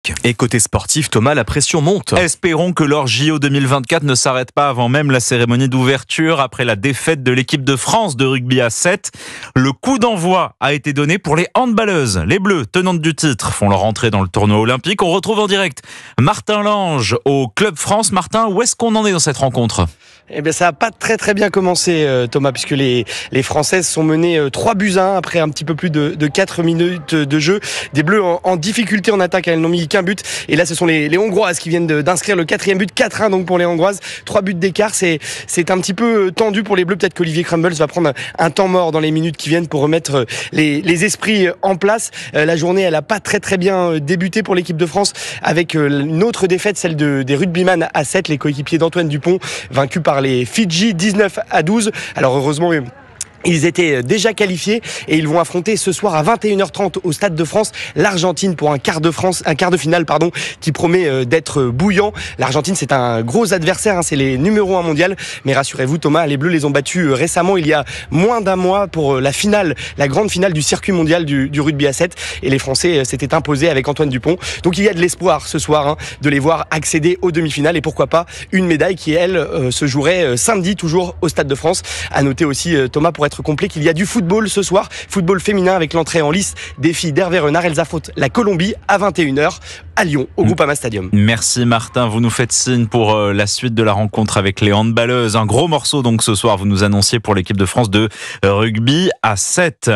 JO Paris 2024 : après leur défaite face aux Fidji, les Bleus du rugby à VII vont disputer leur 1/4
JO Paris 2024 : après leur défaite face aux Fidji, les Bleus du rugby à VII vont disputer leur quart de finale